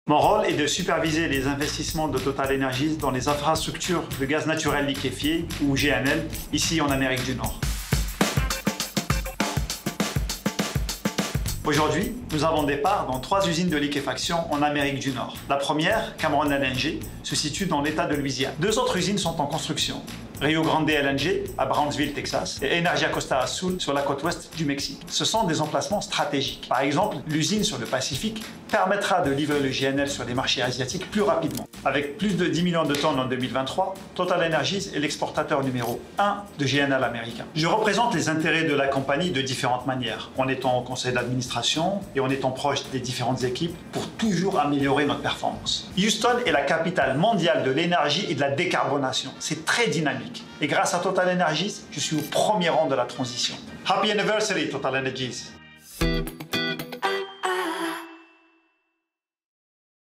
#64 Amine SOUDANI - Liquefied Natural Gas
Meet Amine Soudani, responsible for TotalEnergies' LNG (Liquefied Natural Gas) investments in North America.
100 years, 100 faces. They continue to write TotalEnergies' history.